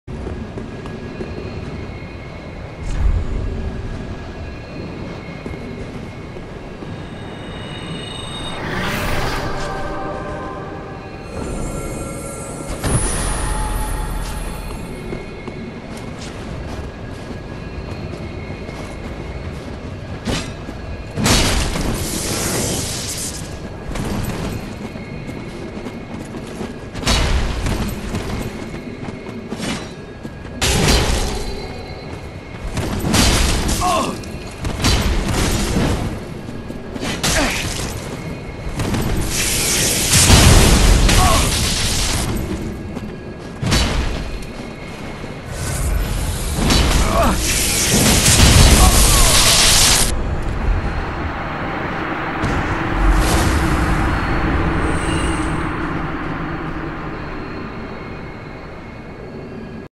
Learning to use lightning stake to go on the offensive against heavy weapons
Level 100